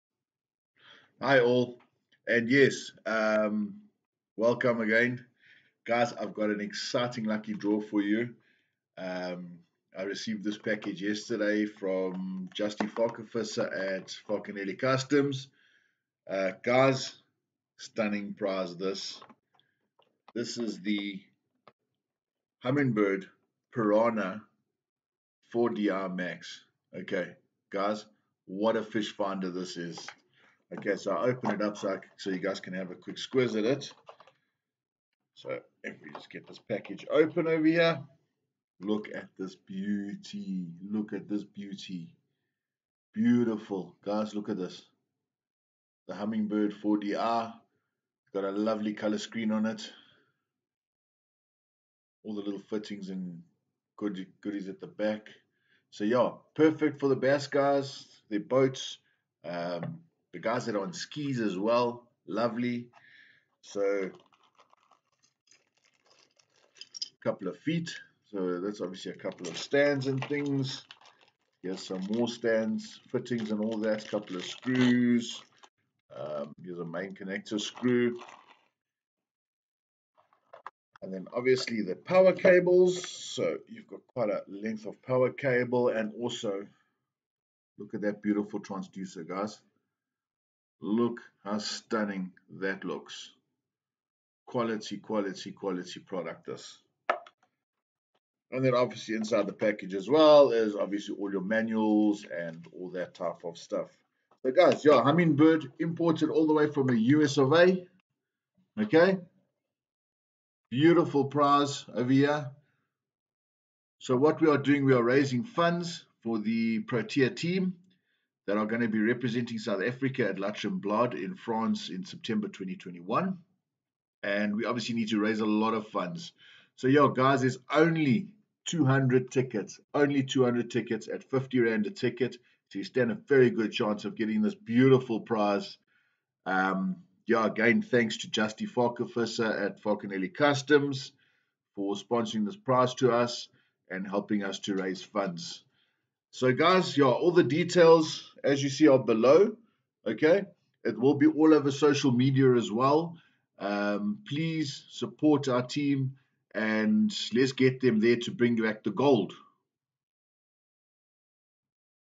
WIN WIN WIN - Humminbird Piranha Max 4 DI Fish Finder
Win this Humminbird Piranha Max 4 DI Fish Finder. Proceeds are to support our Protea Ladies FIPS-M Team to fish the World Championships in La Tremblade France September 2021.

Tickets are only 50 Rands (200 Tickets ONLY)